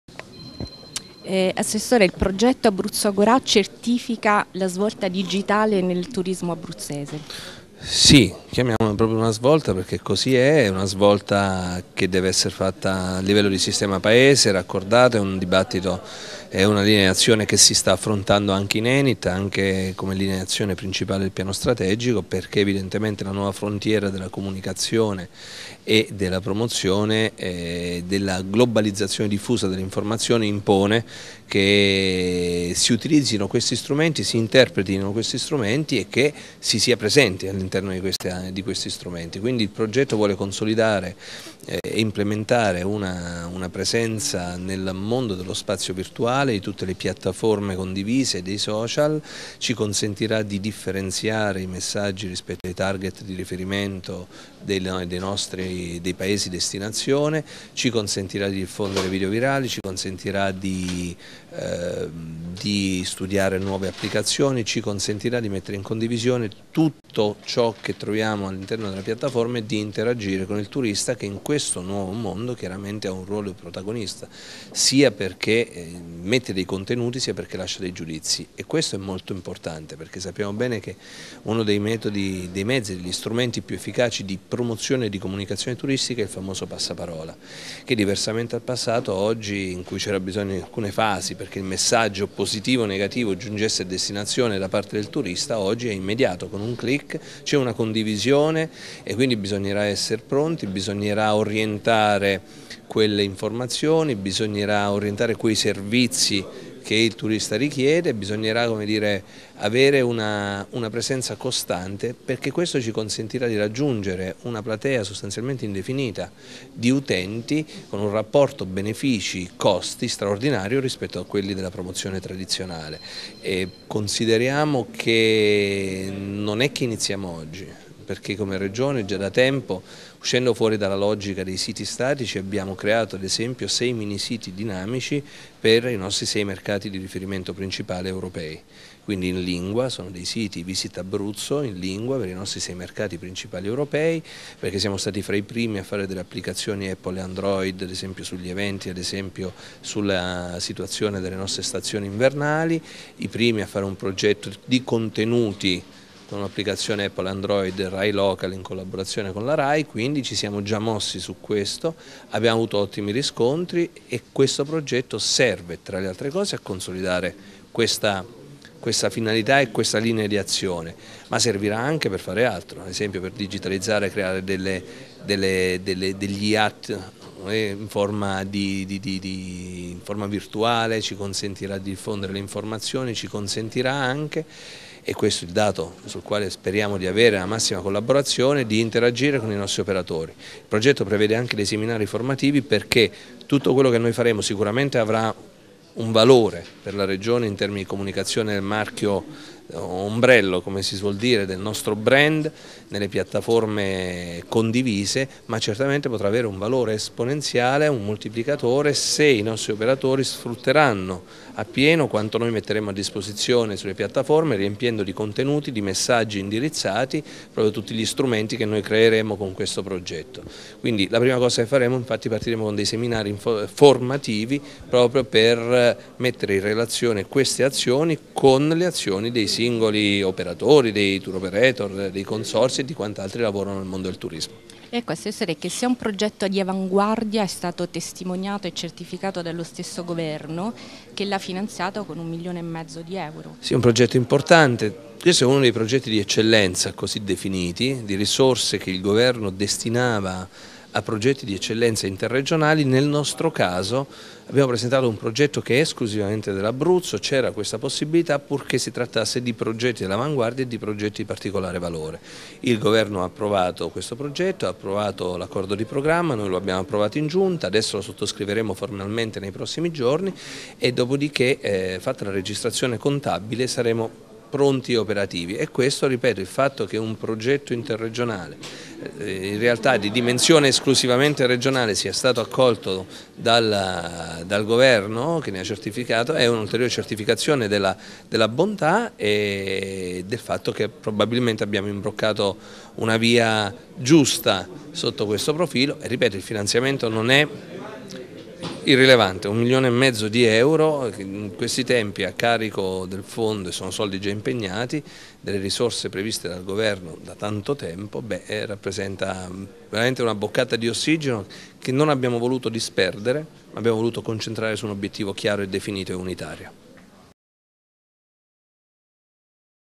Di Dalmazio: Svolta digitale per il turismo in Abruzzo
Il settore Turismo si appresta a vivere una "svolta digitale" nel modo di comunicare la promozione e l'accoglienza, grazie alla creazione della "Piattaforma brand umbrella", che offrira' tutte le informazioni live in arrivo dai vari canali sociali attivati, con una struttura navigabile 2.0, utile alla condivisione di tutti i contenuti. Il progetto, denominato "Abruzzo @gora'", e' stato peraltro ritenuto di "eccellenza turistica" dallo stesso Governo che lo ha cofinanziato con un milione e mezzo di euro. "Abruzzo @gora'" e' stato presentato alla stampa questa mattina dall'assessore alla Promozione turistica, Mauro Di Dalmazio, accanto allo staff tecnico dell'Assessorato. "Il progetto - ha spiegato l'Assessore - pone l'Abruzzo all'avanguardia in primo luogo perche' recepisce con una certa sollecitudine i cambiamenti globali che hanno rivoluzionato le forme di comunicazione e promozione turistica. Non sfugge che oggi la diffusione virale dell'informazione nel mondo del 2.0 e' la forma che moltiplica e modernizza quello che e' sempre stato lo strumento di promozione piu' importante: il passaparola". Citando alcune fonti di studio,Di Dalmazio rivela che l'incidenza del "passaparola" nella scelta delle destinazIoni turistiche ha abbondantemente superato il 30/35 per cento; e che grazie alla capacita' di "esaltare i messaggi negativi e positivi in maniera istantanea, con una diffusione pressocche' totale, rende obbligata la scelta per una regione che voglia consolidare il proprio brand di essere presente su queste piattaforme che, tra l'altro, hanno un rapporto efficacia-costo superiore alle forme classiche di promozione". Poi c'e' il turistica che, nel mondo 2.0, "ha un ruolo centrale perche' lascia giudizi, commenti, espressioni di gradimento che noi andremo a monitorare per intervenire tempestivamente, allo scopo di raggiungere una platea potenzialmente con pochi limiti.
Entrando nel merito del progetto di promozione e accoglienza turistica dell'Abruzzo digitale 2.0, l'assessore Di Dalmazio, ha evidenziato le offerte della piattaforma sia nel "poter inserire messaggi tarati anche sull'obiettivo di utenza potenziale che vogliamo cogliere, sia perche' consente di essere sempre attuali con le informazioni di servizio che devono essere rese". Abruzzo@agora' "non nasce oggi - ha concluso Di Dalmazio - ma e' un progetto in linea con la strategia che l'Assessorato s'e' dato da tempo con applicazioni innovative per fornire informazioni sui bollettini della neve e sugli eventi. Un moltiplicatore dell'efficacia di questi strumenti sara' anche il coinvolgimento degli operatori, per i quali partiranno seminari informativi". Per la operativita' del progetto, dopo gli accordi di programma recepiti prima del Ministero e poi dalla Giunta regionale, resta da attendere la registrazione contabile. Entro febbraio si potra' partire con la svolta digitale. Gli obiettivi del progetto Abruzzo@gora': aumentare la quota nazionale della Destinazione Abruzzo; aggregare e mettere in rete l'offerta turistica abruzzese qualificata, garantendone la visibilita', la comunicazione e promozione del territorio mediante modalita' innovative e servizi all'utente di nuova generazione; destagionalizzare i flussi turistici; utilizzare le caratteristiche differenziali per il posizionamento distintivo sui media; orientare le strategie di differenziazione sui media; migliorare la focalizzazione dell'offerta; migliorare la visibilita' e credibilita' dell'ente. Il progetto prevede la creazione della "Piattaforma brand umbrella", il web marketing, l'ampliamento degli strumenti social, la creazione di App e blog tematici, la realizzazione di seminari informativi, l'implementazione di gadget elettronici e games interattivi, la realizzazione di video promozionali, la creazione dei corrispettivi nella navigazione mobile